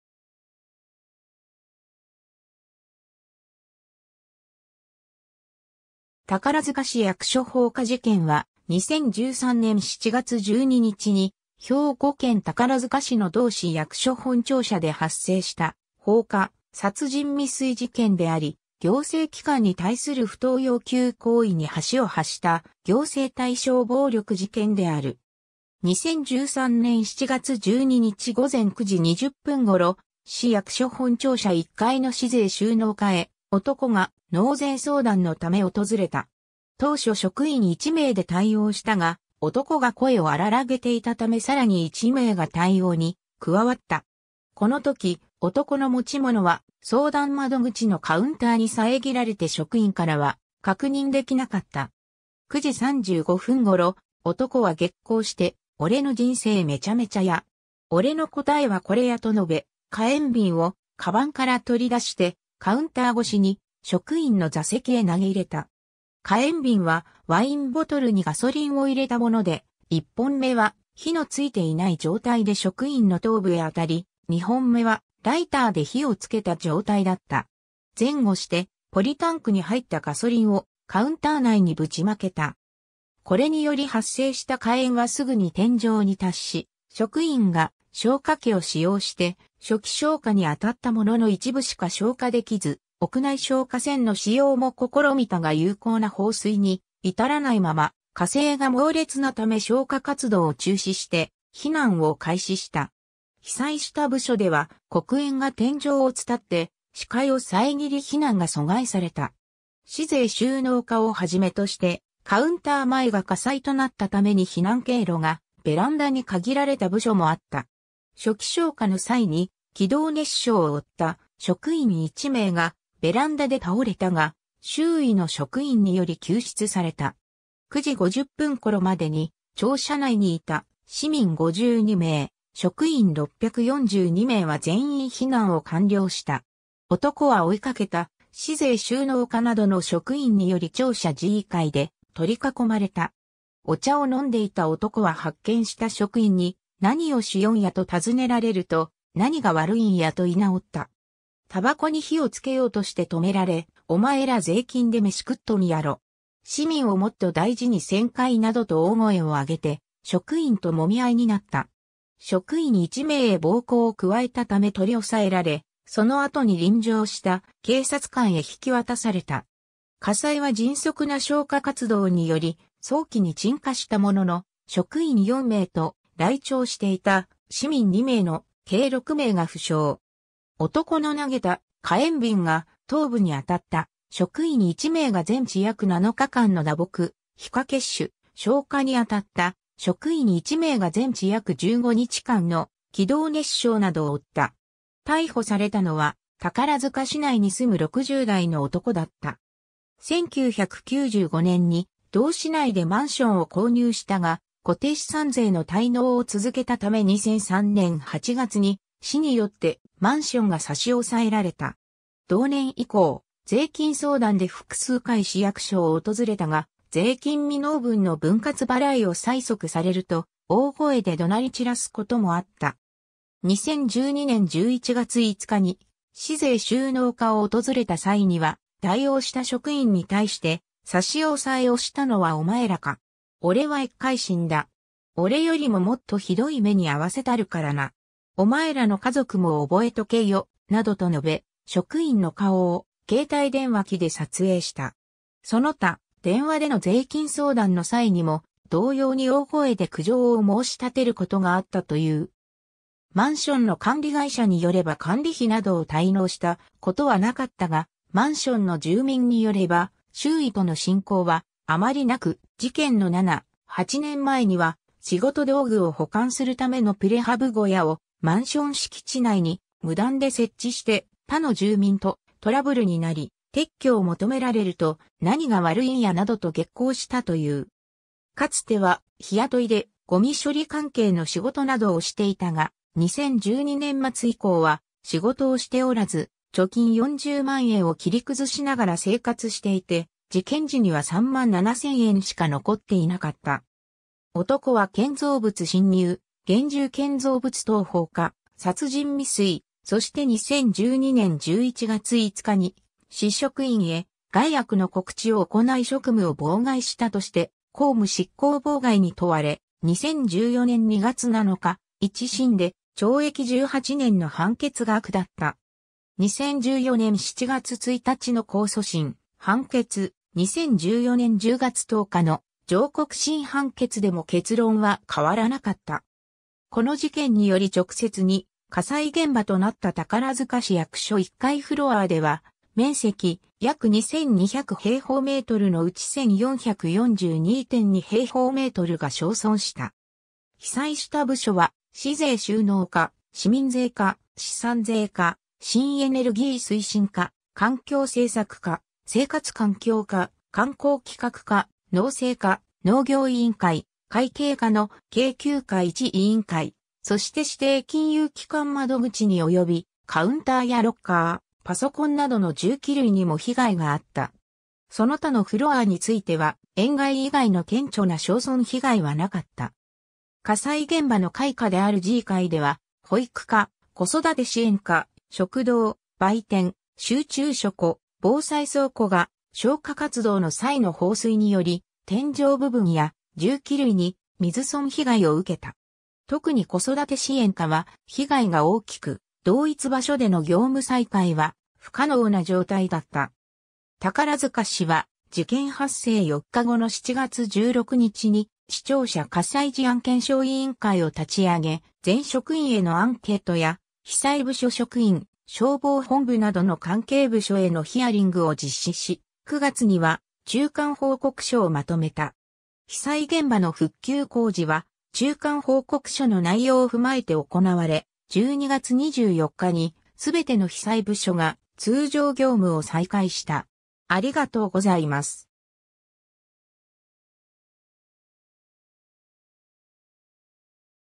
宝塚市役所放火事件
宝塚市役所放火事件（たからづかしやくしょほうかじけん）は、2013年7月12日に兵庫県宝塚市の同市役所本庁舎で発生した放火・殺人未遂事件であり、行政機関に対する不当要求行為に端を発した行政対象暴力事件である。
 2013年7月12日午前9時20分頃、市役所本庁舎1階の市税収納課へ男が納税相談のため訪れた。
当初職員1名で対応したが、男が声を荒らげていたためさらに1名が対応に加わった。
この時、男の持ち物は相談窓口のカウンターに遮られて職員からは確認できなかった。
9時35分頃、男は激高して「おれの人生めちゃめちゃや。おれの答えはこれや」と述べ、火炎瓶をかばんから取り出してカウンター越しに職員の座席へ投げ入れた。
火炎瓶はワインボトルにガソリンを入れたもので、1本目は火の付いていない状態で職員の頭部へ当たり、2本目はライターで火を付けた状態だった。
前後してポリタンクに入ったガソリンをカウンター内にぶちまけた。
 これにより発生した火炎はすぐに天井に達し、職員が消火器を使用して初期消火に当たったものの一部しか消火できず、屋内消火栓の使用も試みたが有効な放水に至らないまま、火勢が猛烈なため消火活動を中止して避難を開始した。
被災した部署では黒煙が天井を伝って視界を遮り避難が阻害された。
市税収納課を始めとして、カウンター前が火災となったために避難経路がベランダに限られた部署もあった。
初期消火の際に気道熱傷を負った職員1名がベランダで倒れたが、周囲の職員により救出された。
9時50分頃までに庁舎内にいた市民52名、職員642名は全員避難を完了した。
 男は追いかけた市税収納課などの職員により庁舎G階（グラウンドフロアー、地上階）で取り囲まれた。
お茶を飲んでいた男は発見した職員に「何をしよんや」と尋ねられると「何が悪いんや」と居直った。
たばこに火を付けようとして止められ「おまえら税金で飯食っとんやろ。市民をもっと大事にせんかい」などと大声を上げて職員ともみ合いになった。
職員1名へ暴行を加えたため取り押さえられ、その後に臨場した警察官へ引き渡された。
 火災は迅速な消火活動により早期に鎮火したものの、職員4名と来庁していた市民2名の計6名が負傷（中等症から軽症）。
男の投げた火炎瓶が頭部に当たった職員1名が全治約7日間の打撲・皮下血腫、消火に当たった職員1名が全治約15日間の気道熱傷などを負った。
 逮捕されたのは宝塚市内に住む60代の男だった。
1995年に同市内でマンションを購入したが、固定資産税の滞納を続けたため2003年8月に市によってマンションが差し押さえられた。
同年以降、税金相談で複数回市役所を訪れたが、税金未納分の分割払いを催促されると大声で怒鳴り散らすこともあった。
2012年11月5日に市税収納課を訪れた際には、対応した職員に対して「差押えをしたのはお前らか。俺は１回死んだ。俺よりももっとひどい目にあわせたるからな。お前らの家族も覚えとけよ」などと述べ、職員の顔を携帯電話機で撮影した。
その他、電話での税金相談の際にも同様に大声で苦情を申し立てることがあったという。
 マンションの管理会社によれば管理費などを滞納したことはなかったが、マンションの住民によれば周囲との親交はあまりなく、事件の7、8年前には仕事道具を保管するためのプレハブ小屋をマンション敷地内に無断で設置して他の住民とトラブルになり、撤去を求められると「何が悪いんや」などと激高したという。
かつては日雇いでごみ処理関係の仕事などをしていたが、2012年末以降は仕事をしておらず、貯金40万円を切り崩しながら生活していて、事件時には3万7000円しか残っていなかった。
 男は建造物侵入、現住建造物等放火、殺人未遂、そして2012年11月5日に市職員へ害悪の告知を行い職務を妨害したとして公務執行妨害に問われ、2014年2月7日、一審で懲役18年（求刑懲役21年）の判決が下った。
2014年7月1日の控訴審判決、2014年10月10日の上告審判決でも結論は変わらなかった。
 この事件により直接に火災現場となった宝塚市役所1階フロアーでは、面積約2200平方メートル（階段部等を含む）のうち1442.2平方メートルが焼損した。
被災した部署は市税収納課、市民税課、資産税課、新エネルギー推進課、環境政策課、生活環境課、観光企画課、農政課、農業委員会、会計課の計9課1委員会、そして指定金融機関窓口に及び、カウンターやロッ...